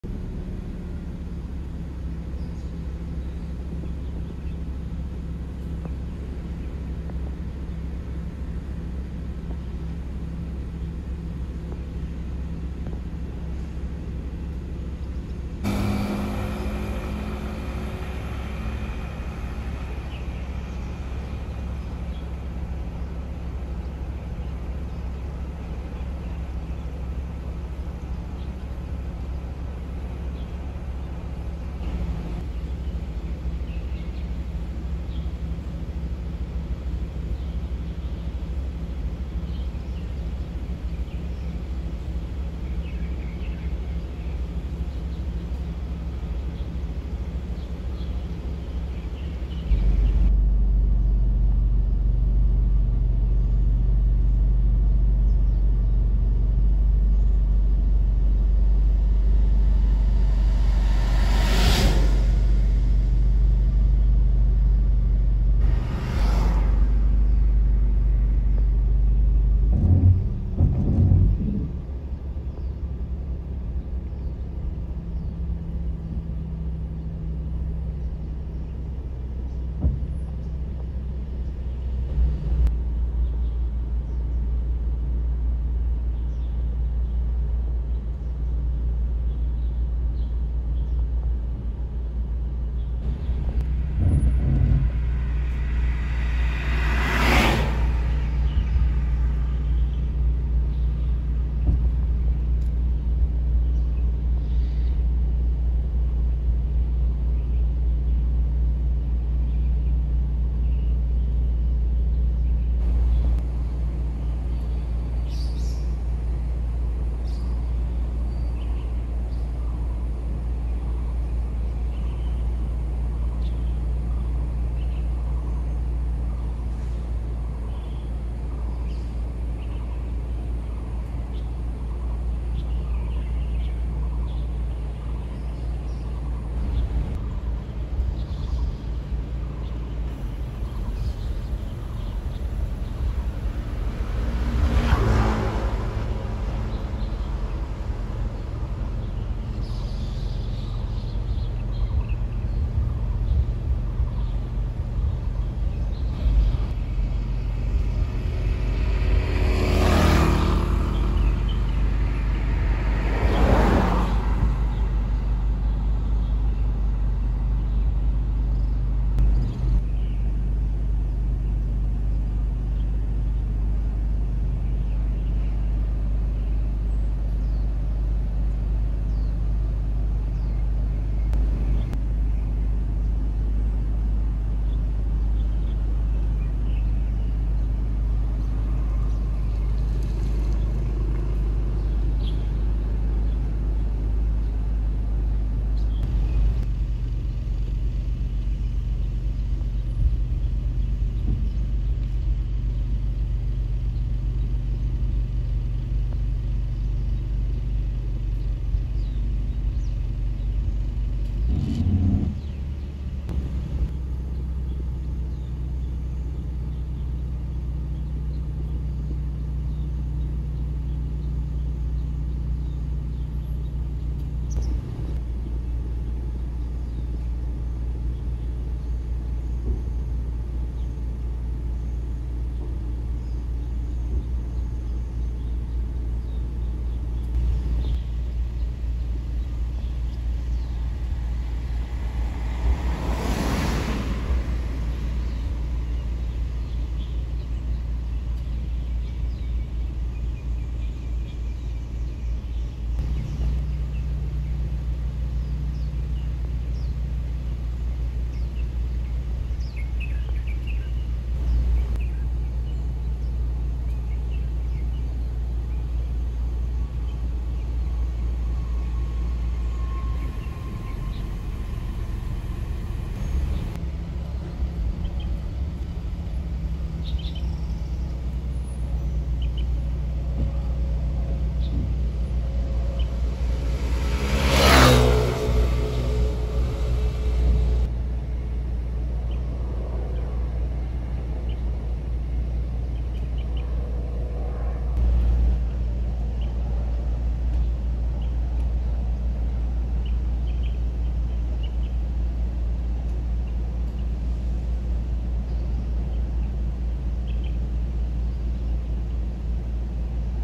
台鐵 和平車站 台泥水泥貨列 車庫 機關車
2020.4.2 台鐵 和平車站 台灣水泥貨列 車庫 機關車